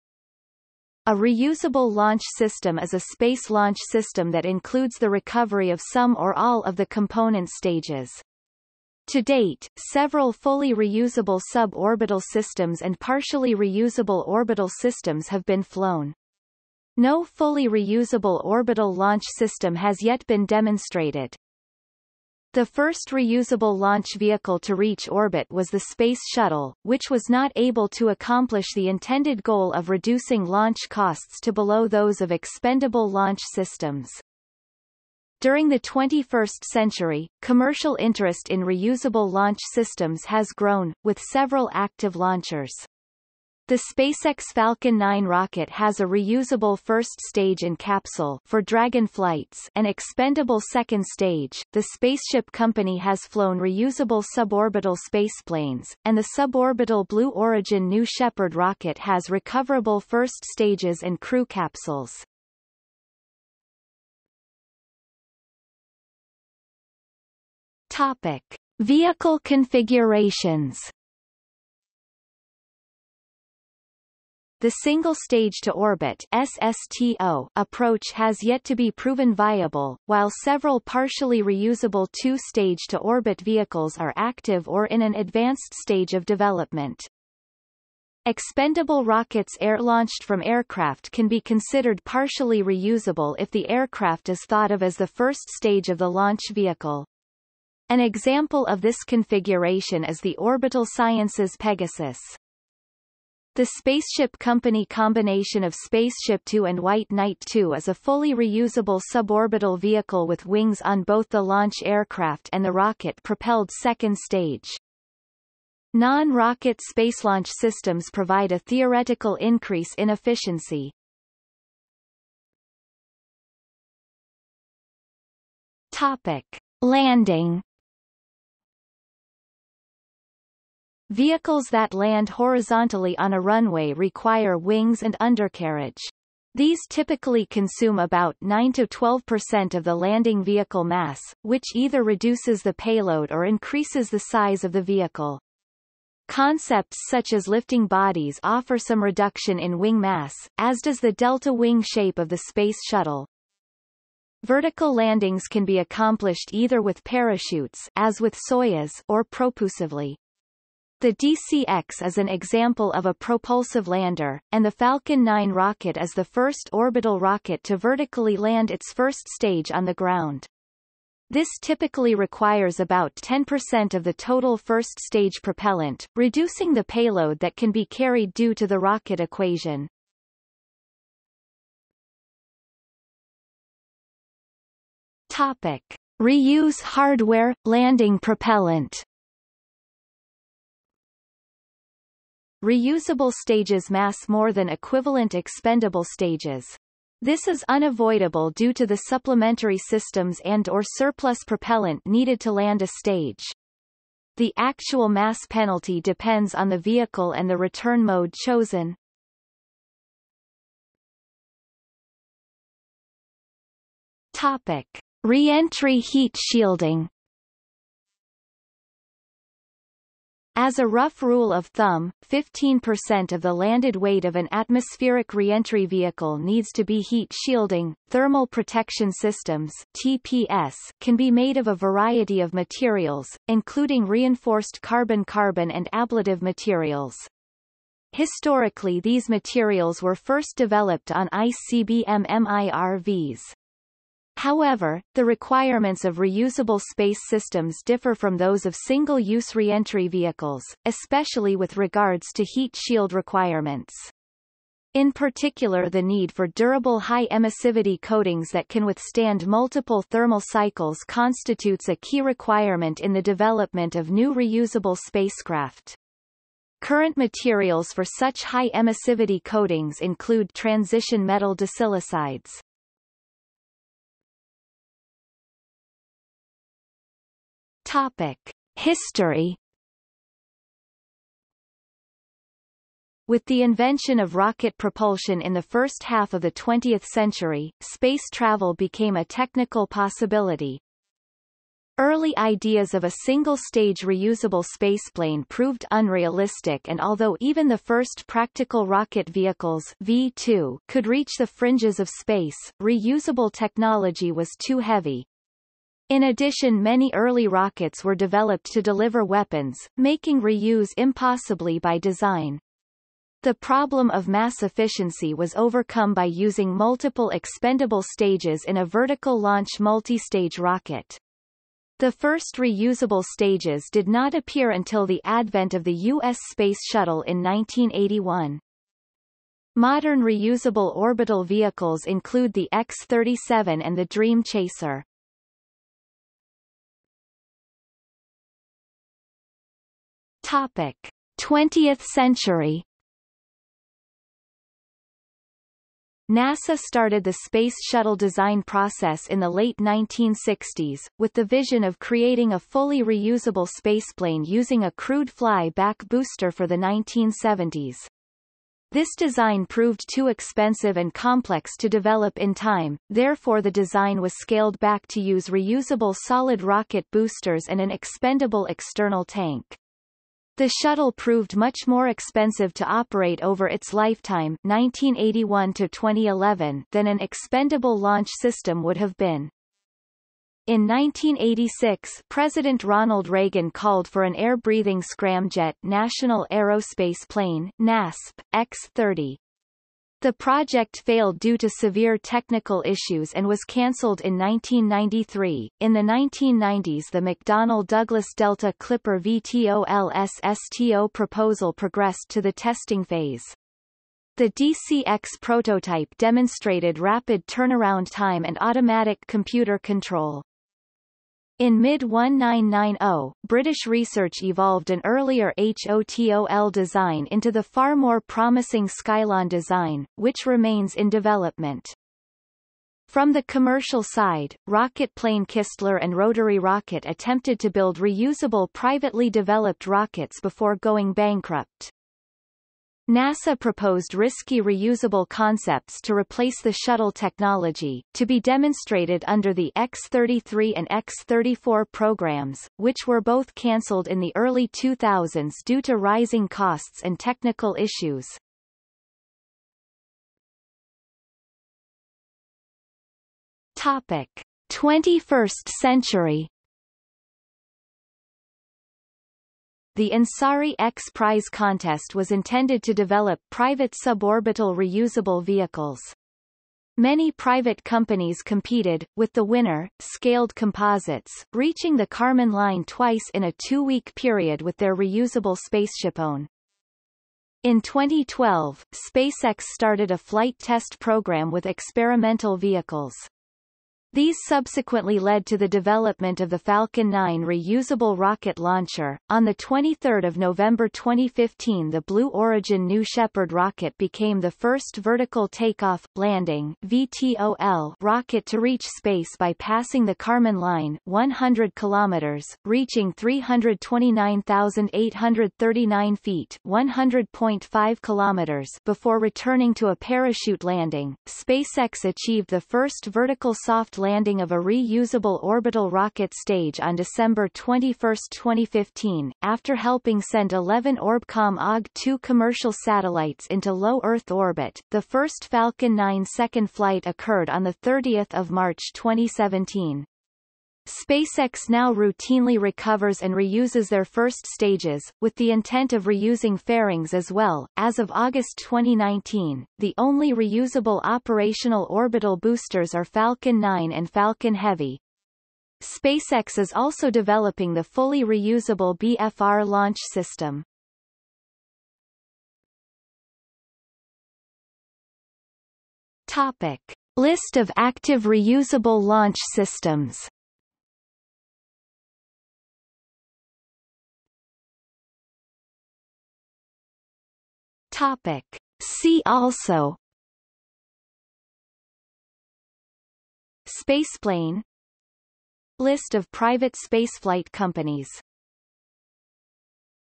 Reusable launch vehicle | Wikipedia audio article
00:01:04 1 Vehicle configurations
00:02:02 1.1 Landing
00:03:01 1.2 Reuse hardware/landing propellant
00:03:30 1.3 Reentry heat shielding
00:04:34 2 History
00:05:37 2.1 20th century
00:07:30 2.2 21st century
00:09:12 3 List of active reusable launch systems
00:09:24 4 See also

- Socrates

SUMMARY
A reusable launch system is a space launch system that includes the recovery of some or all of the component stages. To date, several fully reusable sub-orbital systems and partially reusable orbital systems have been flown. No fully reusable orbital launch system has yet been demonstrated.
The first reusable launch vehicle to reach orbit was the Space Shuttle, which was not able to accomplish the intended goal of reducing launch costs to below those of expendable launch systems.
During the 21st century, commercial interest in reusable launch systems has grown, with several active launchers. The SpaceX Falcon 9 rocket has a reusable first stage and capsule (for Dragon flights) and expendable second stage, The Spaceship Company has flown reusable suborbital spaceplanes, and the suborbital Blue Origin New Shepard rocket has recoverable first stages and crew capsules.